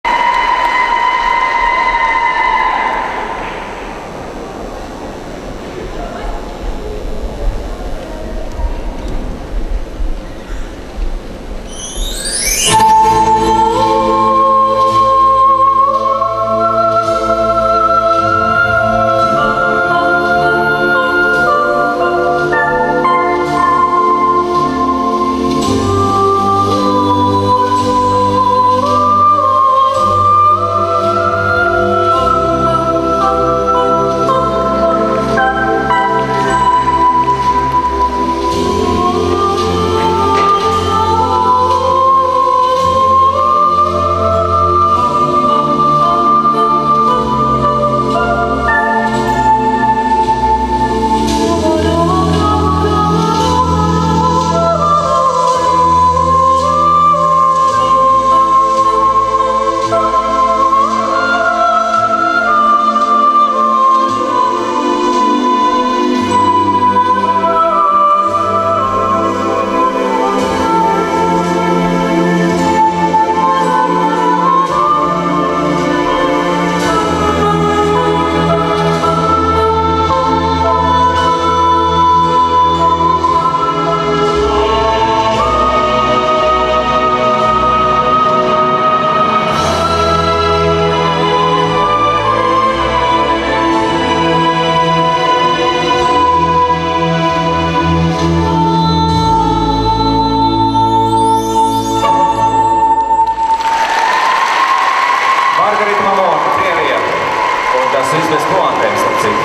МАРГАРИТА МАМУН ПОКАЗАТЕЛЬНЫЕ BALTIJAS APLIS 2012
ПОКАЗАТЕЛЬНЫЕ ВЫСТУПЛЕНИЯ УЧАСТНИЦ КОНКУРСА ПО ХУДОЖЕСТВЕННОЙ ГИМНАСТИКЕ BALTIJAS APLIS 2012